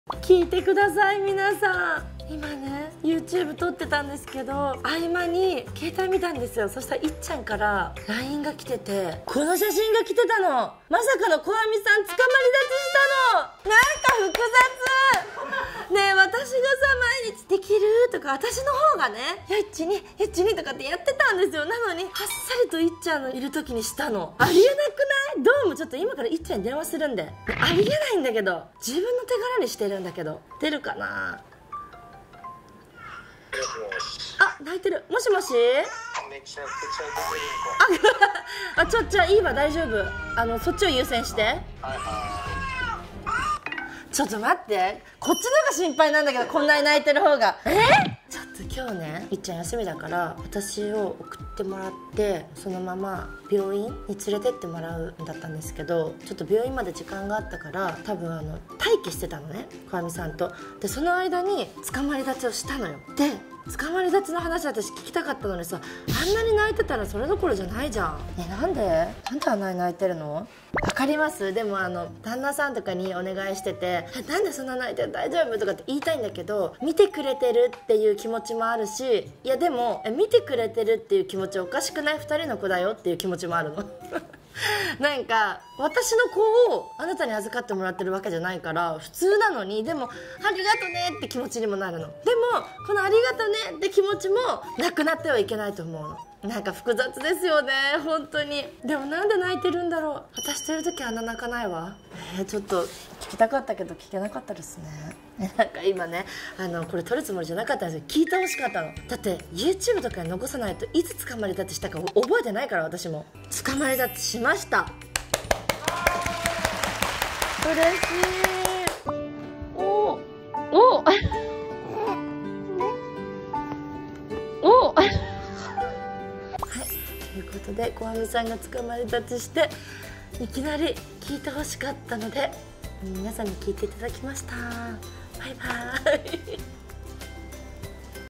【ちょっとみんな聞いて】祝 つかまり立ち！こあみちゃんが立ったよ！
こんばんは！
あみちゃんねるスタッフです🐣

収録中に突然の連絡が！
なんと、こあみちゃんが始めてつかまり立ちをしました！

みんなに見て欲しくて緊急動画です！

【YouTubeについてのお仕事・お問合せ先はこちら】
FIREBUG